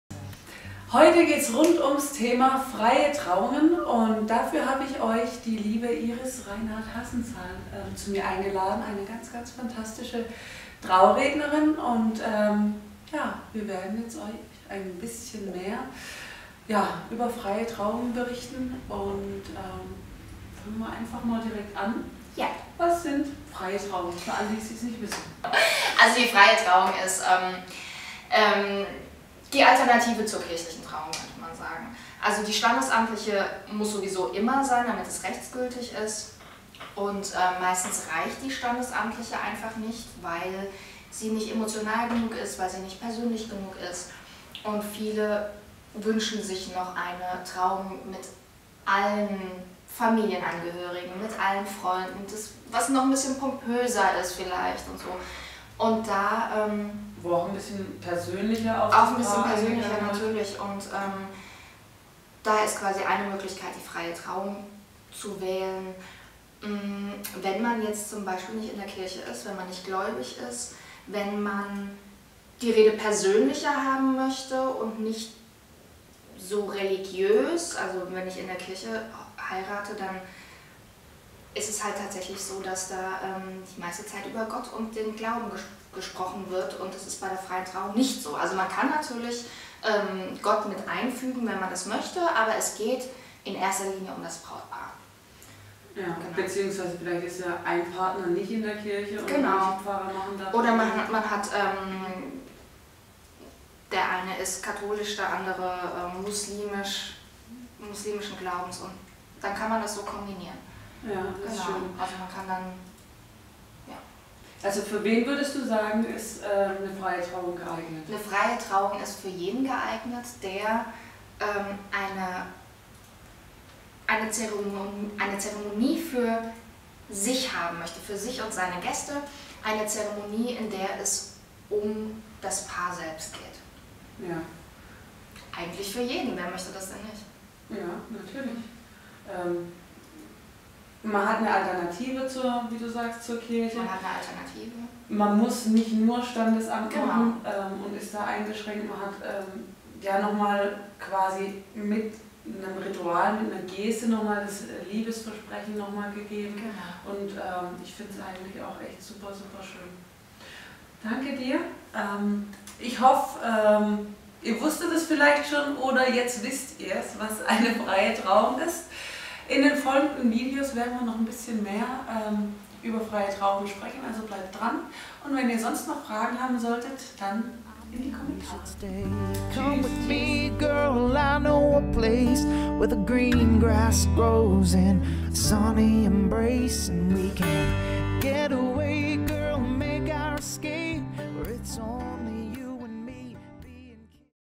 HOCHZEITSEINMALEINS - FREIE TRAUUNG - Was ist das? - Traurednerin Reinhardt Hassenzahl
♥♥♥ HOCHZEITSEINMALEINS -  FREIE TRAUUNG - Was ist das? -  ♥♥♥

Eine freie Trauung ist eine wunderbare Alternative zur kirchlichen Trauung beziehungsweise bietet allen Brautpaaren zusätzlich zum Standesamt eine schöne Zeremonie mit einer ganz persönlichen Traurede, die ganz auf die Wünsche und Vorstellungen des Brautpaares abgestimmt sind, ob in einem feierlichen Rahmen oder ungezwungen in der freien Natur. Die freie Trauung kann sowohl weltlich als auch mit religiösen Elementen abgehalten werden und prinzipiell ist eine freie Trauung überall möglich.
Iris Reinhard Hassenzahl ist eine fantastische Traurednerin und erzählt uns in unser aktuellen Interviewserie mit ihr mehr über das Thema freien Trauung.

Viel Spaß beim Anschauen. Falls ihr Fragen haben solltet, könnt ihr uns gerne in die Kommentare schreiben oder eine Nachricht über unsere Website hinterlassen.

Alles Liebe